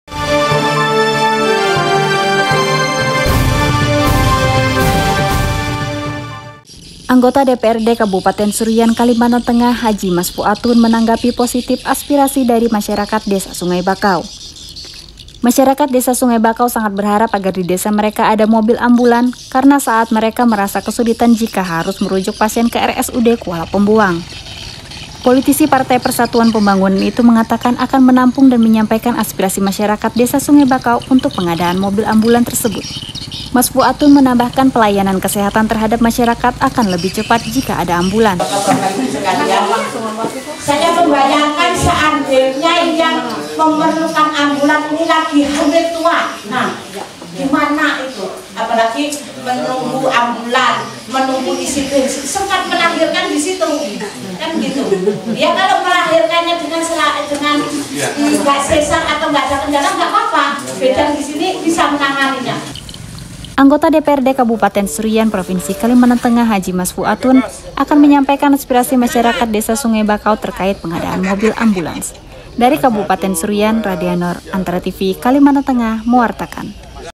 DPRD Seruyan tampung usulan masyarakat terkait pengadaan mobil ambulans
ANTARA - Anggota DPRD Kabupaten Seruyan, Kalimantan Tengah Hj. Masfuatun menanggapi positif usulan masyarakat Sungai Bakau terkait pengadaan mobil ambulans. (Radianor/Redianto Tumon/Rini Andriani).